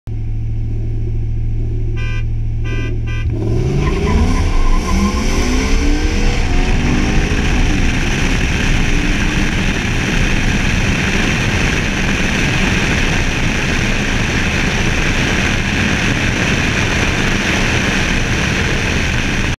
05 c6 corvette ls2 stock vs intake exhaust 5.0
Dig 1/8 unmarked off 3rd honk